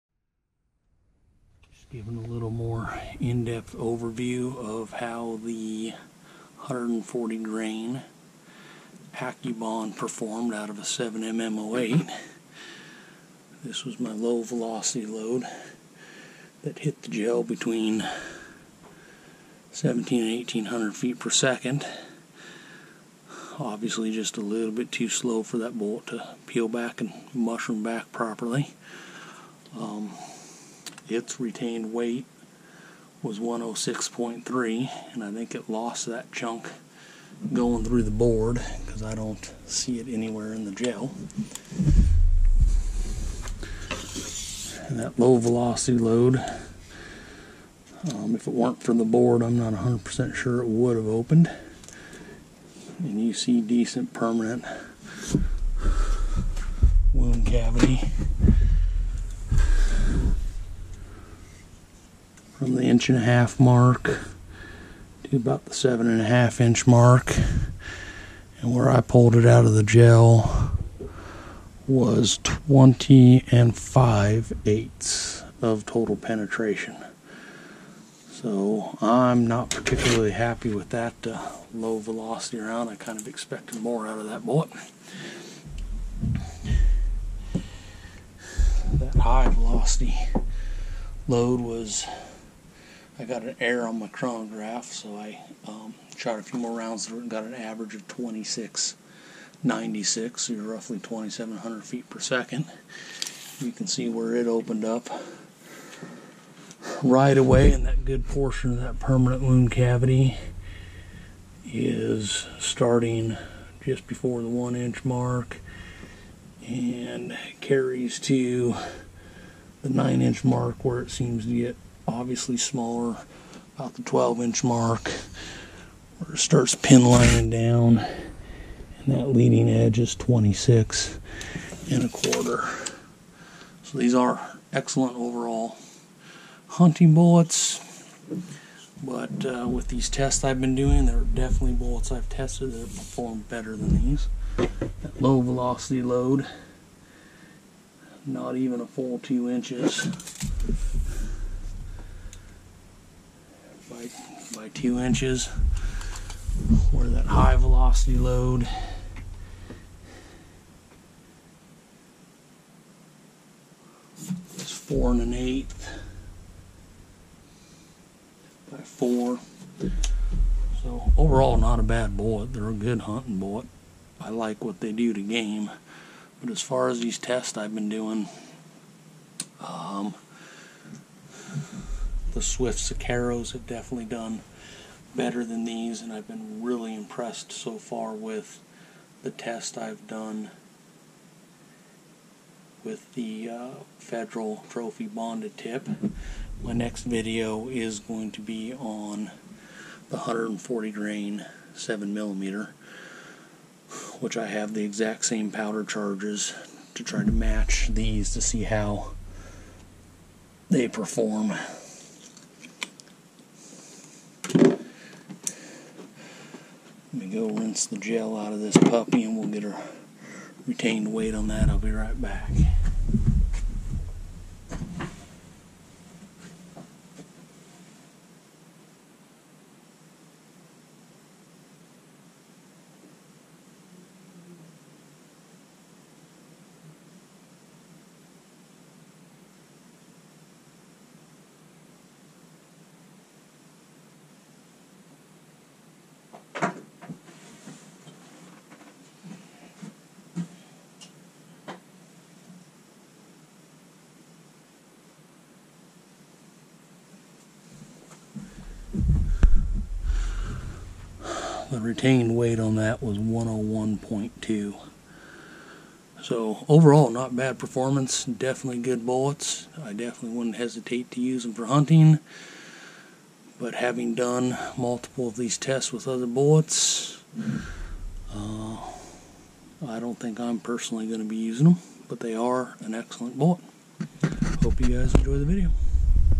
7mm-08 140 gr Nosler Accubond overview of ballistic gel
More in depth look at how the Nosler Accubond 140 gr .284 bullet performed ballistically shot out of a 7mm-08 into 10% ballistics gelatin. Overall not bad performance. I thought they would hold together a little better then they did. Not a great weight retention for being a bonded bullet. I'm not sure if that has to do with the thickness of the jacket the lead alloy or both.

Low velocity round 1746 fps and high was 2700 ish fps. I had a error reading on the high velocity round and my average shooting a string through the chronograph was 2696 fps. Weight retention was 106.3 gr on the low velocity and 101.2 on the high velocity.